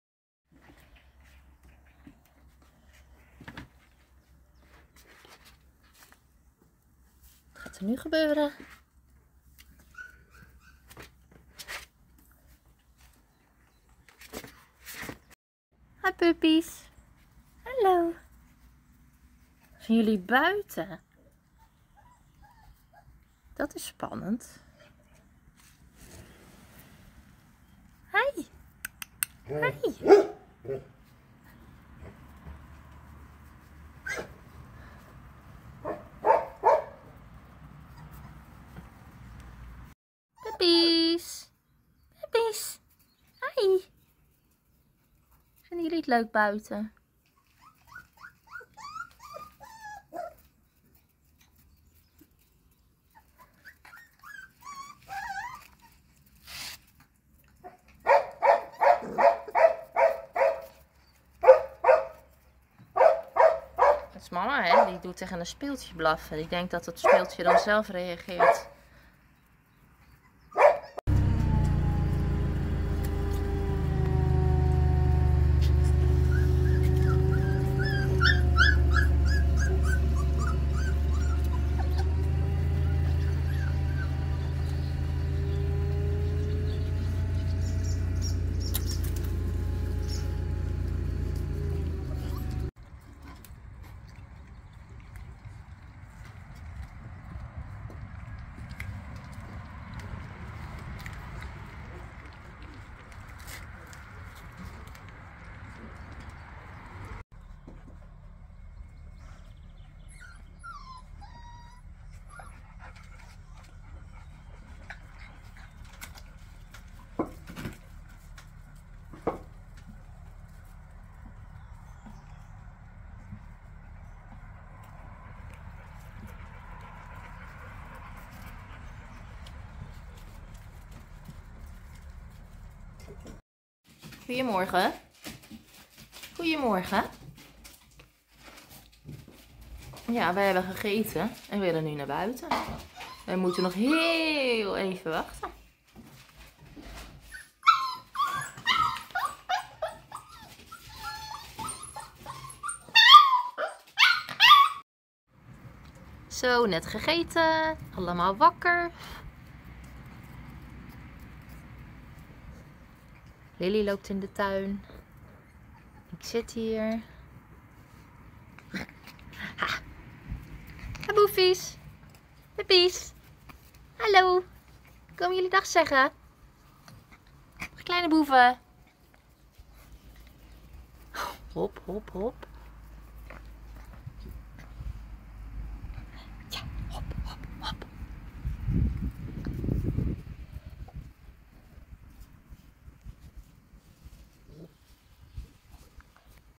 4 weken oud! Naar buiten!
De pups van onze Golden Retrievers Rose en Lily zijn nu 4 weken oud en mogen naar buiten.